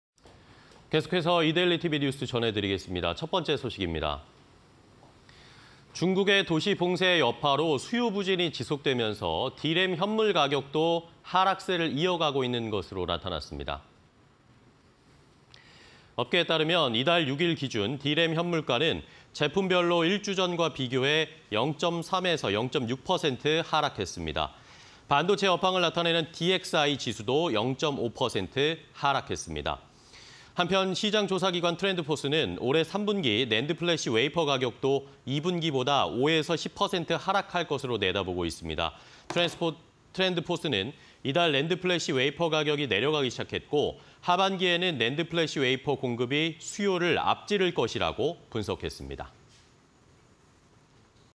중국 '도시 봉쇄'에 수요 타격...D램 현물가 하락세
중국의 도시 봉쇄 여파로 수요 부진이 지속되면서 D램 현물가격도 하락세를 이어가고 있는 것으로 나타났습니다.
업계에 따르면 이달 6일 기준 D램 현물가는 제품별로 1주 전과 비교해 0.3%∼0.6% 하락했습니다. 반도체 업황을 나타내는 DXI 지수도 0.5% 하락했습니다.
한편 시장조사기관 트렌드포스는 올해 3분기 낸드플래시 웨이퍼 가격도 2분기보다 5∼10% 하락할 것으로 내다보고 있습니다.
트렌드포스는 "이달 낸드플래시 웨이퍼 가격이 내려가기 시작했고, 하반기에는 낸드플래시 웨이퍼 공급이 수요를 앞지를 것"이라고 분석했습니다.